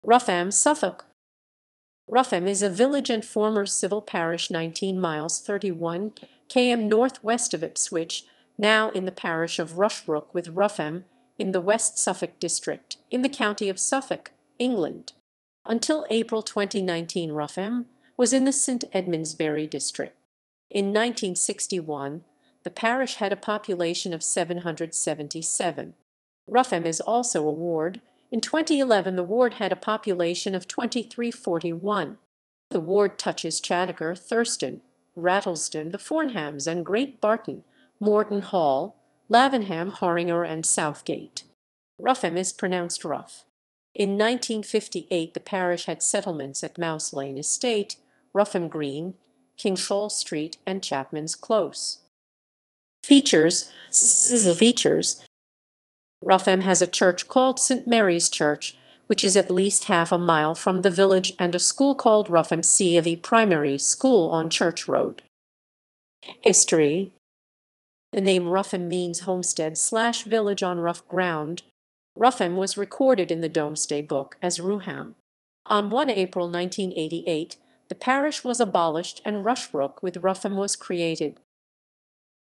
Rougham, Suffolk
Rougham is a village and former civil parish 19 miles (31 km) north west of Ipswich, now in the parish of Rushbrooke with Rougham, in the West Suffolk district, in the county of Suffolk, England. Until April 2019 Rougham was in the St Edmundsbury district. In 1961 the parish had a population of 777. Rougham is also a ward, in 2011 the ward had a population of 2341. The ward touches Chadacre, Thurston, Rattlesden, The Fornhams & Great Barton, Moreton Hall, Lavenham, Horringer and Southgate. Rougham is pronounced "Ruff'm". In 1958 the parish had settlements at Mouse Lane estate, Rougham Green, Kingshall Street and Chapmans Close.